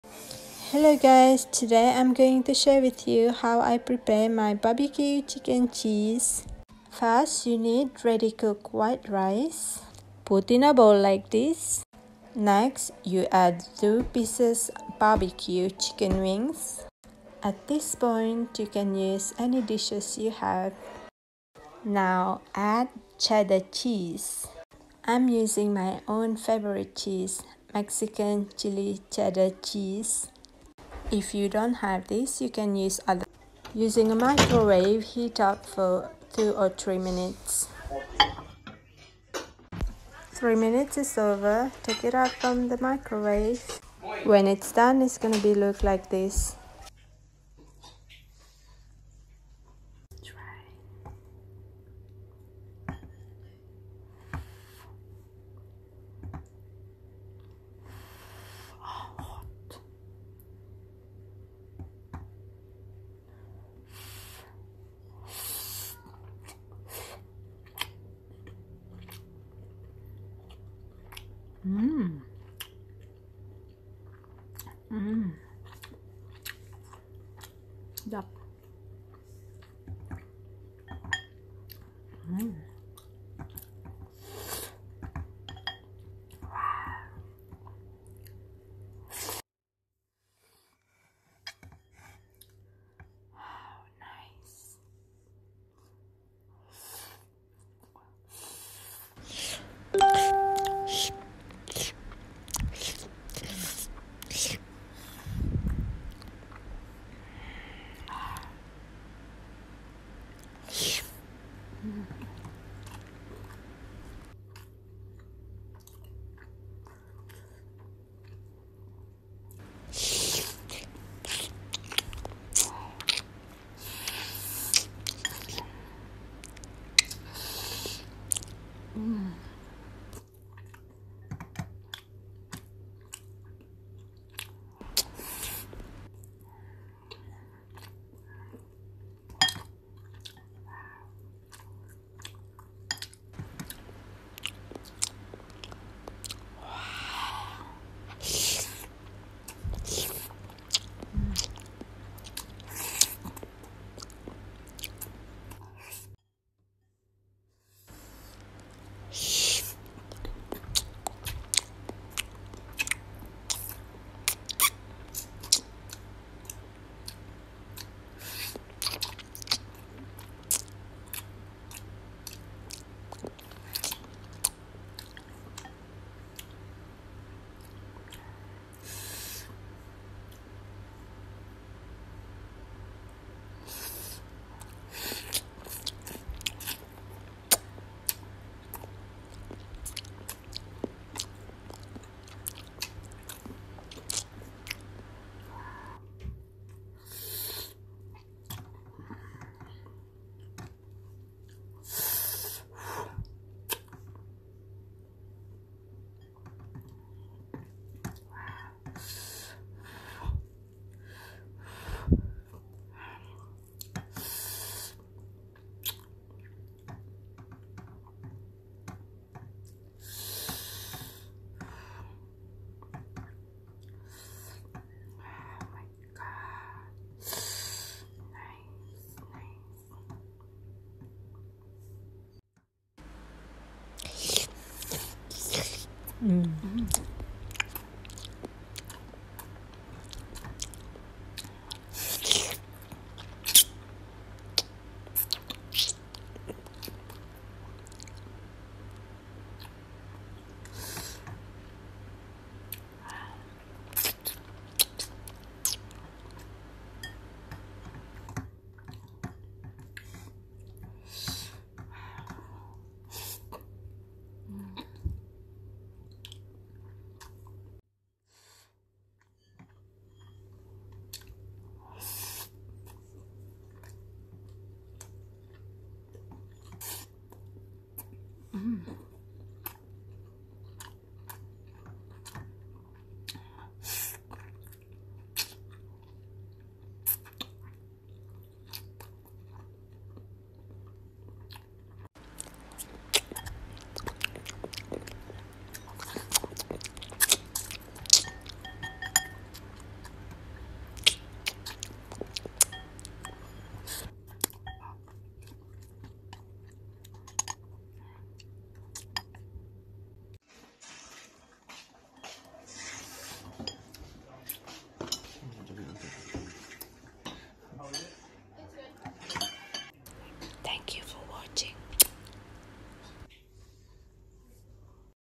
BBQ Chicken wing with Mexican cheddar cheese
Easy and super delicious self creation food.. perfect for urban life style.
I rather make and eat at home 😝